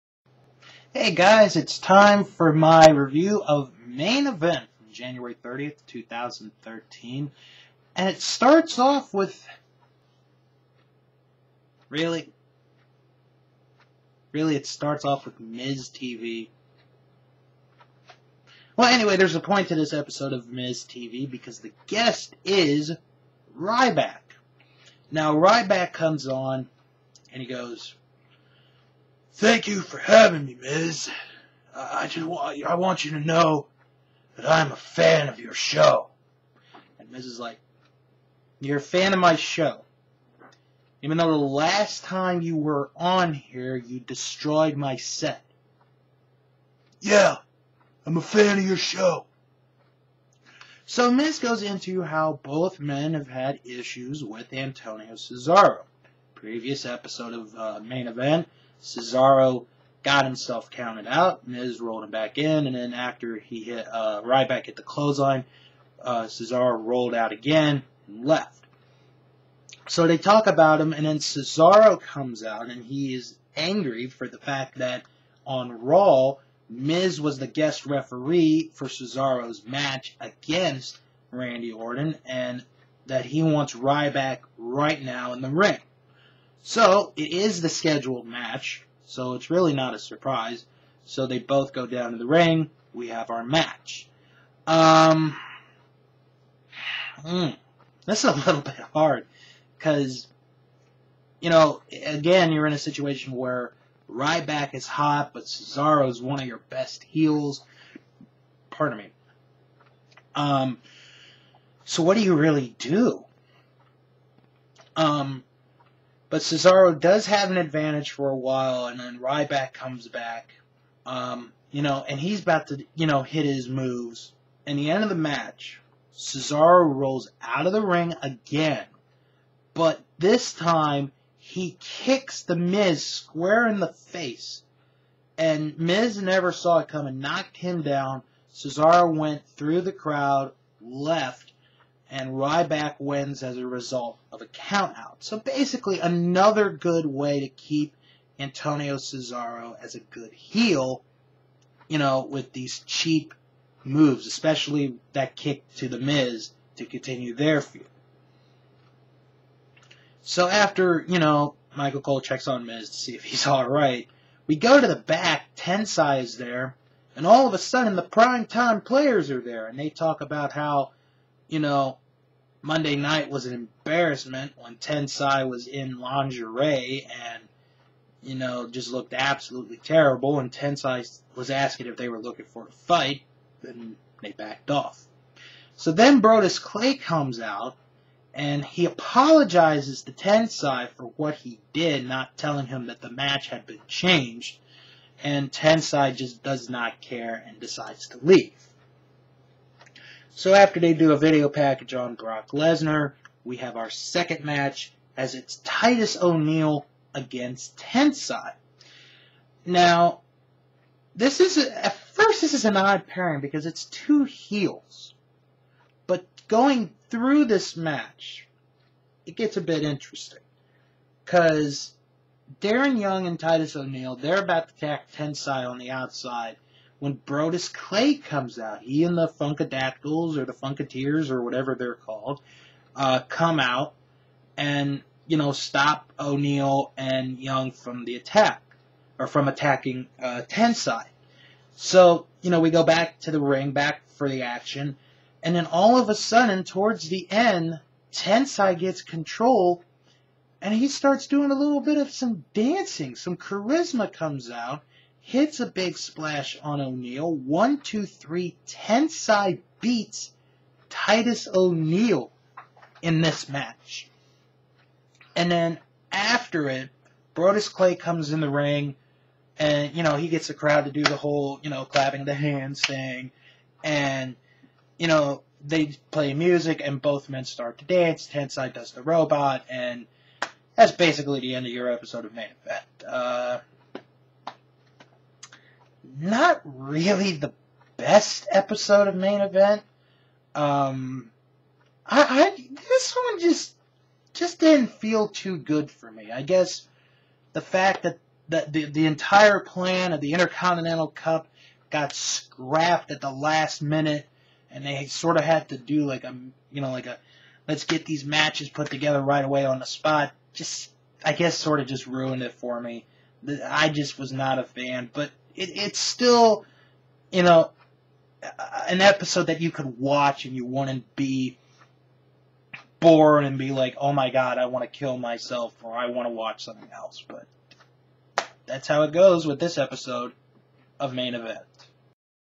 WWE Main Event 1/30/13 Review
It's another Ryback vs. Antonio Cesaro match, boy what a thrill that is.